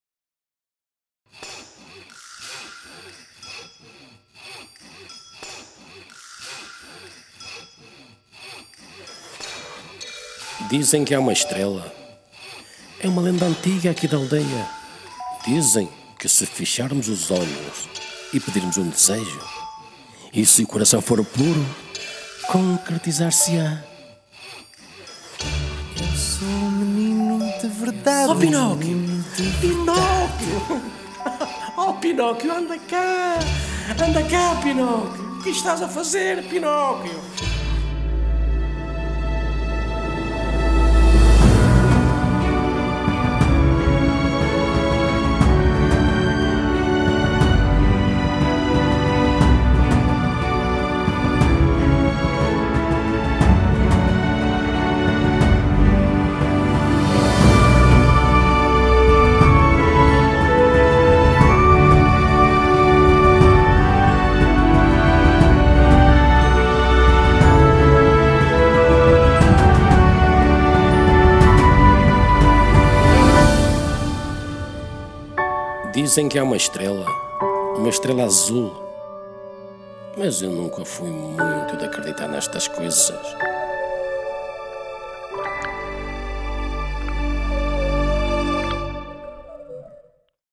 Jangada Teatro- "Pinóquio" (Trailer Oficial)
Pinóquio | 2020 | 53ª Produção

A partir do conto de  Carlo Collodi

Dramaturgia | Filipe Gouveia
Encenação | Xico Alves

Interpretação | Filipe Gouveia, Sara Maia, Sónia Ribeiro, Paulo Pires,
Vítor Fernandes e Xico Alves

Música Original e Desenho de Som | Paulo Pires
Figurinos | Cláudia Ribeiro
Cenografia | Carlos Pinheiro e Emanuel Santos (Coletivo Monte)
Desenho e Construção de Marioneta e Máscaras | Sandra Neves (Coletivo Monte)

Desenho e Operação de Luz | Fernando Oliveira
Operação de Som| Fred Meireles

Design Gráfico | Zé Tavares
Fotografias de Cena | Filipa Brito
Vídeo | Chromia studio

Produção Executiva | Alejandrina Romero, Fred Meireles e Susana Morais

Classificação Etária: M/6
Duração: 60 min aprox.